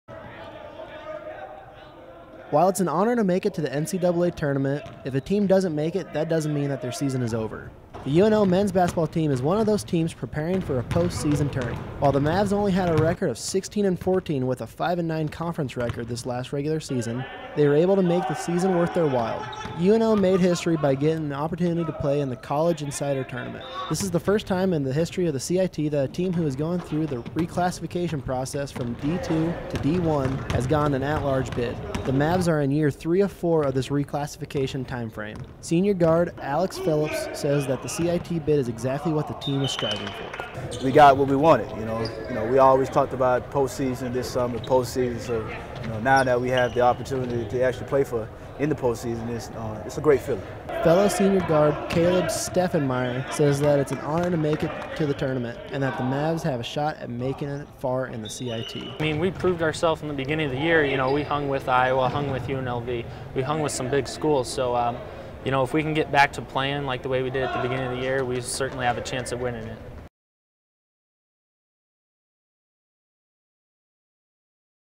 Kantor Package4 UNO Basketball
A preview of UNO Men's Basketball as they prepare to enter the CollegeInsider.com Tournament